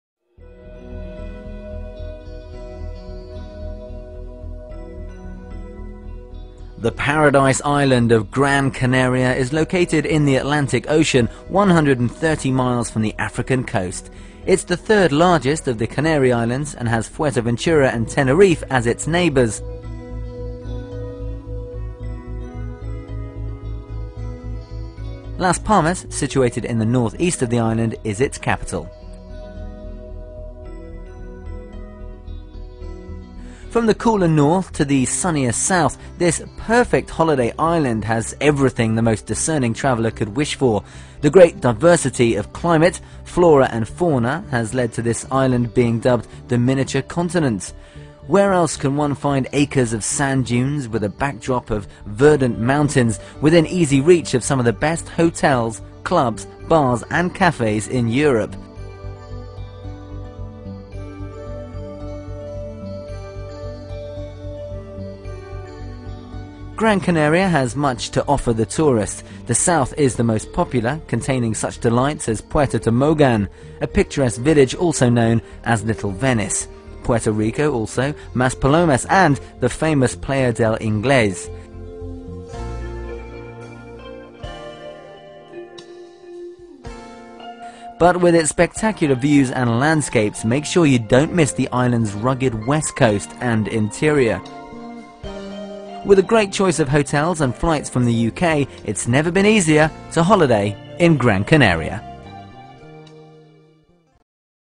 Gran Canaria Holiday Guide
A useful destination guide to the holiday island of Gran Canaria courtesy of Worldwide Timeshare Hypermarket and The Timeshare Lord Blog.